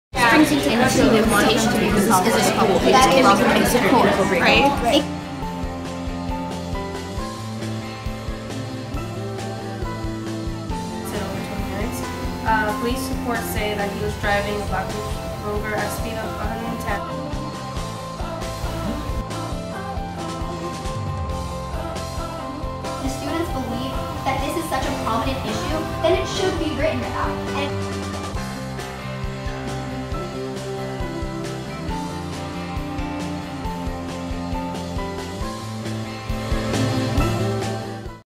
FSPA District 7 Conference 2017
SPJ Florida board and chapter members attended the conference to host a number of sessions for middle and high school journalists.